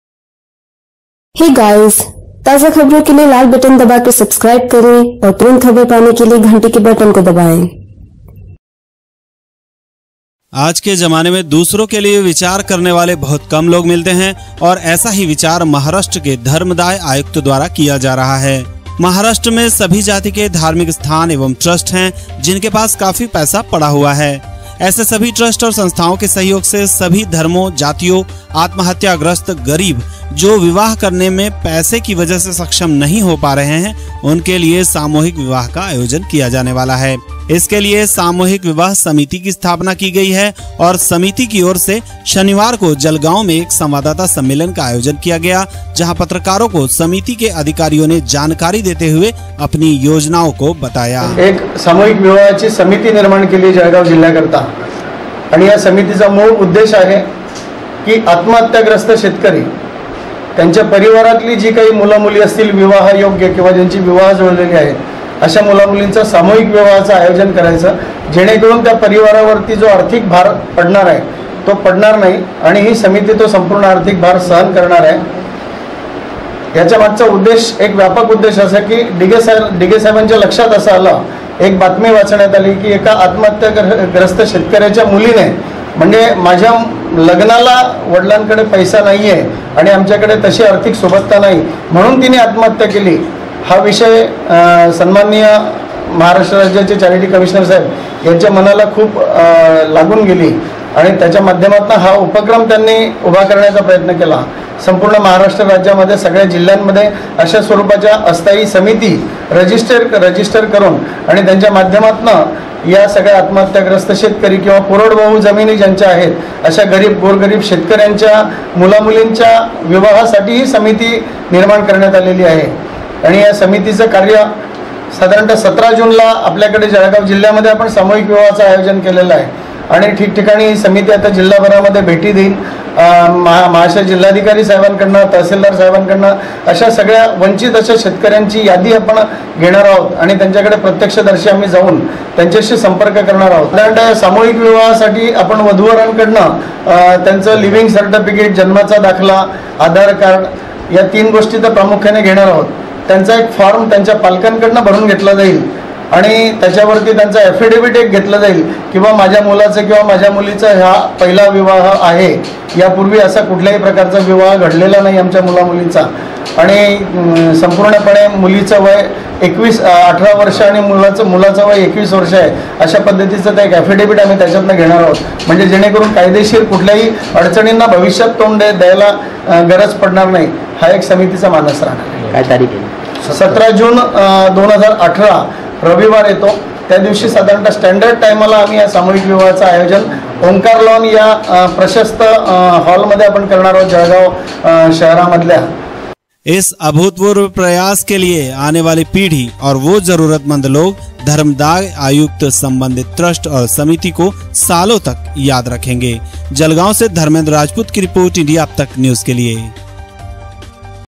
आत्महत्या ग्रस्त ,गरीब,को धर्मदाय आयुक्त द्वारा सामूहिक विवाह का आयोजन
ऐसे सभी ट्रस्ट और संस्थाओ के सहयोग से सभी धर्मो जातियों , आत्महत्या ग्रस्त , गरीब,जो विवाह करने में पैसे की वजह से सक्षम नहीं हो पा रहे उनके लिए सामूहिक विवाह का आयोजन किया जाने वाला है !